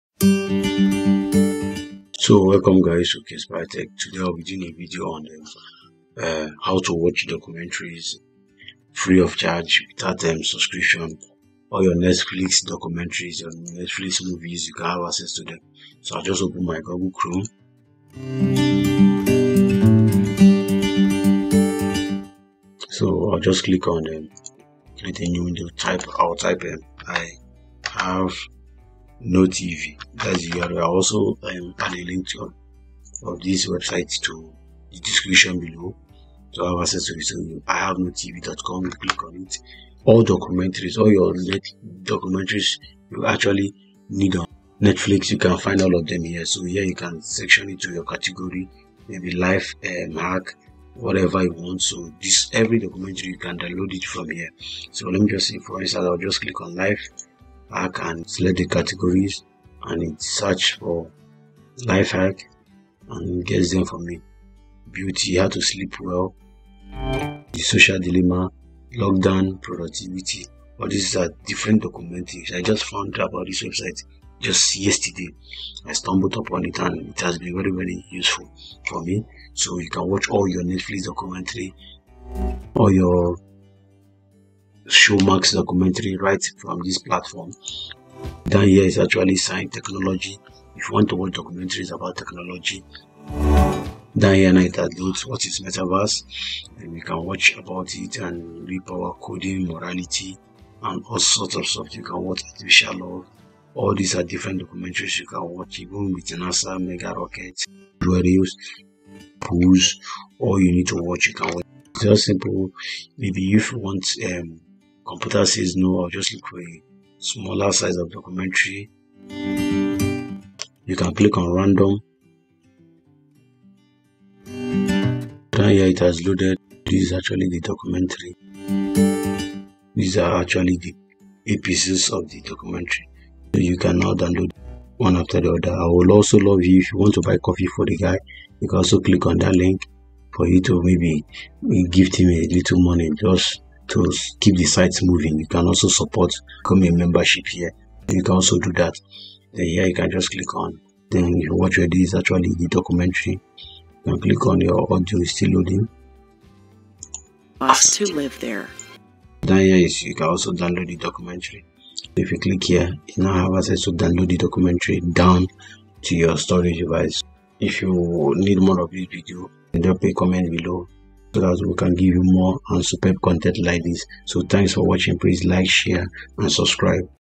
IHaveNoTV | Alternative To Netflix For Free Documentaries
iHaveNoTV.com is described as 'Free Documentaries Website - Astronomy, Nature, Brain, Science, Health, Lifehack' and is a Video Streaming app in the video & movies category.